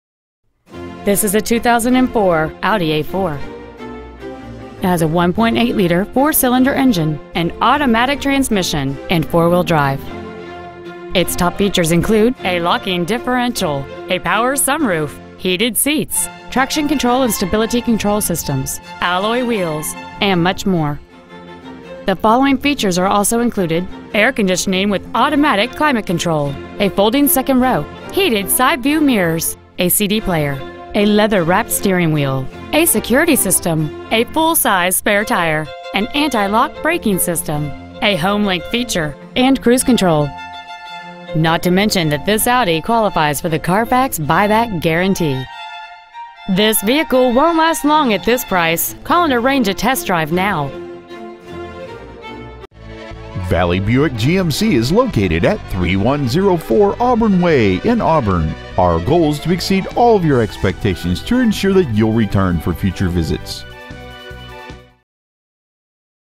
Pre-Owned 2004 Audi A4 Auburn WA
Year : 2004
 Make : Audi
 Model : A4
 Engine : 1.8L I4 Turbo
 Trans . : Automatic
 Exterior : Silver
 Miles : 108,297
 Interior : Gray
 Stock : 12100